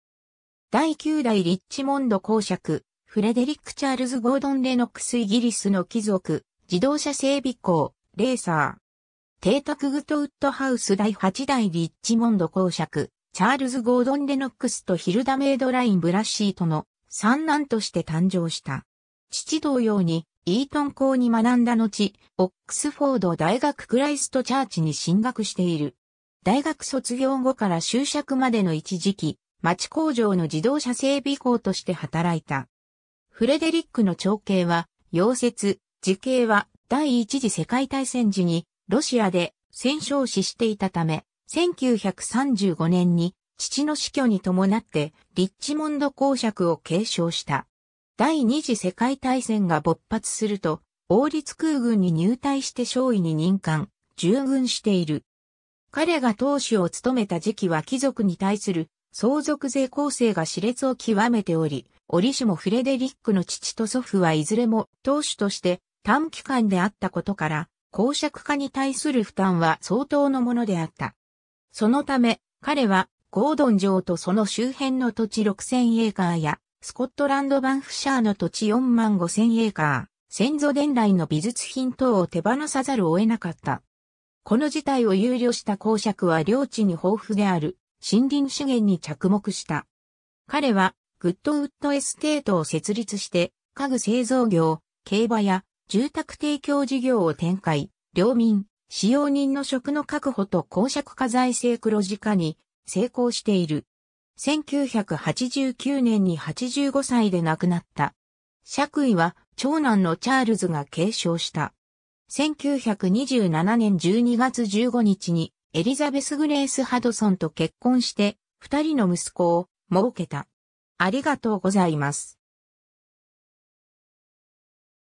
フレデリック・ゴードン＝レノックス (第9代リッチモンド公爵)
第9代リッチモンド公爵フレデリック・チャールズ・ゴードン＝レノックス（英語: Frederick Charles Gordon Lennox, 9th Duke of Richmond、1904年2月5日 - 1989年11月2日）イギリスの貴族、自動車整備工、レーサー。
 邸宅グッドウッド・ハウス 第8代リッチモンド公爵チャールズ・ゴードン＝レノックスとヒルダ・メイドライン・ブラッシー（Hilda Madeline Brassey、1872年6月16日 - 1971年12月29日、庶民院議員ヘンリー・ブラッシーの娘）との三男として誕生した。
 父同様にイートン校に学んだのち、オックスフォード大学クライストチャーチに進学している。
大学卒業後から襲爵までの一時期、町工場の自動車整備工として働いた。
フレデリックの長兄は夭折、次兄は第一次世界大戦時にロシアで戦傷死していたため、1935年に父の死去に伴って、リッチモンド公爵を継承した。
 第二次世界大戦が勃発すると、王立空軍に入隊して少尉に任官、従軍している。
 彼が当主を務めた時期は貴族に対する相続税攻勢が熾烈を極めており、折しもフレデリックの父と祖父はいずれも当主として短期間であったことから、公爵家に対する負担は相当のものであった。
そのため、彼はゴードン城とその周辺の土地6000エーカーやスコットランド・バンフシャーの土地45000エーカー、先祖伝来の美術品等を手放さざるを得なかった。
 この事態を憂慮した公爵は領地に豊富である森林資源に着目した。
彼はグッドウッド・エステートを設立して、家具製造業、競馬や住宅提供事業を展開、領民、使用人の職の確保と公爵家財政黒字化に成功している。
 1989年に85歳で亡くなった。
爵位は長男のチャールズが継承した。
 1927年12月15日にエリザベス・グレイス・ハドソン（Elizabeth Grace Hudson、1992年没、トマス・ウィリアム・ハドソンの娘）と結婚して、2人の息子をもうけた。